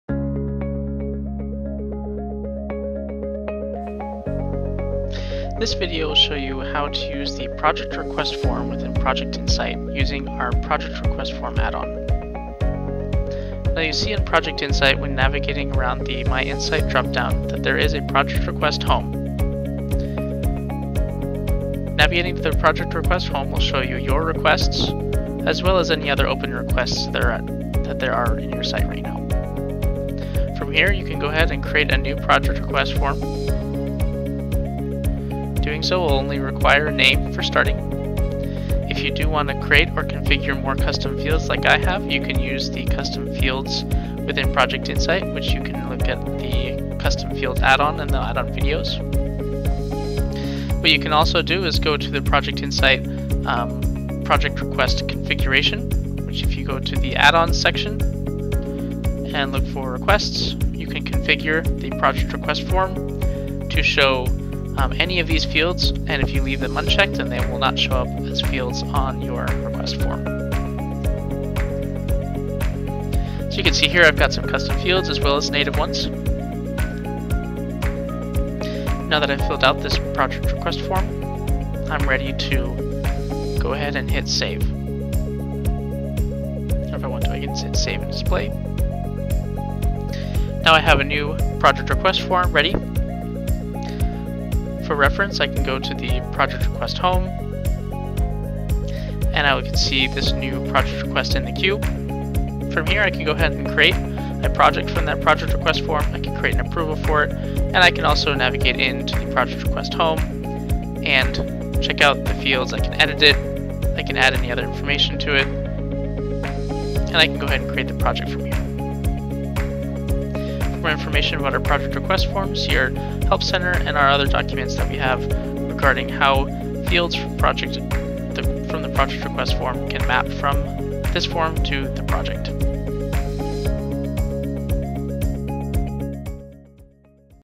Project Request Add-On
Save time and standardize your processes with the Project Request feature. This feature lets you route project requests for approval and then automatically create and assign projects once they are approved.